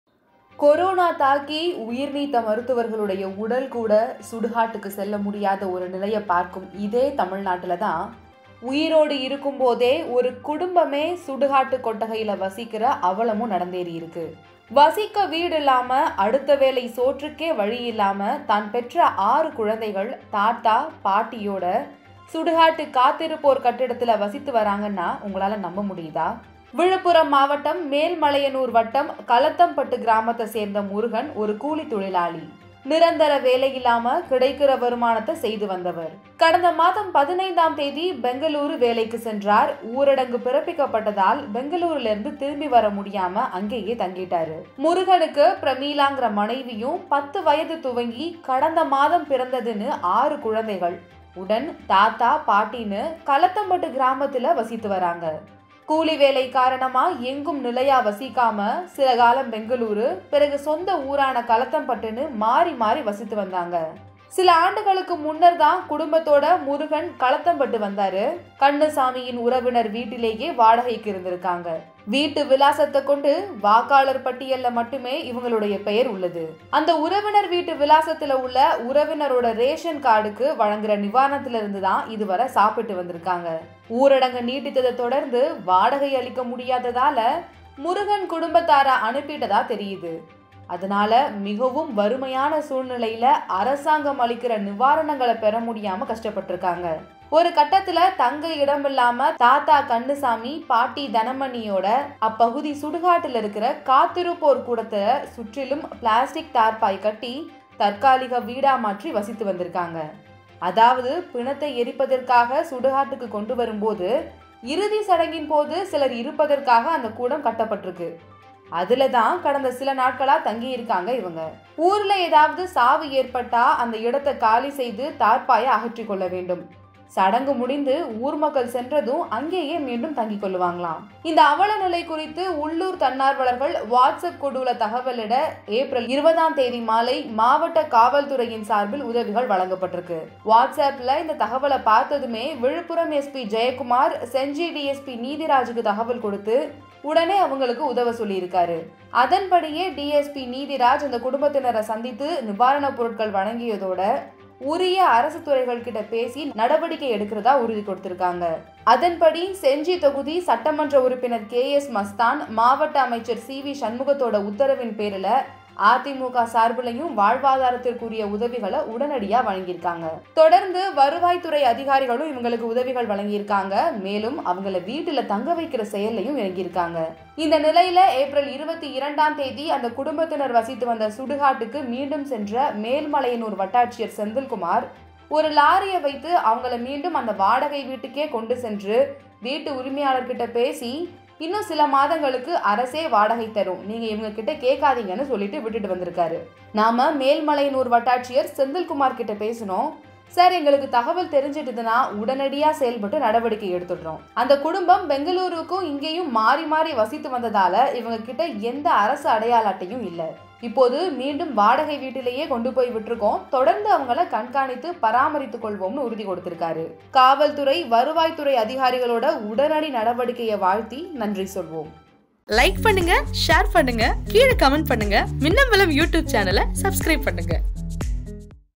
TN Govt. saved the family who lived in the Cemetery! | Minnambalam.com
சுடுகாட்டில் வசித்த குடும்பம்: காப்பாற்றிய அரசு!

Helo App: Minnambalam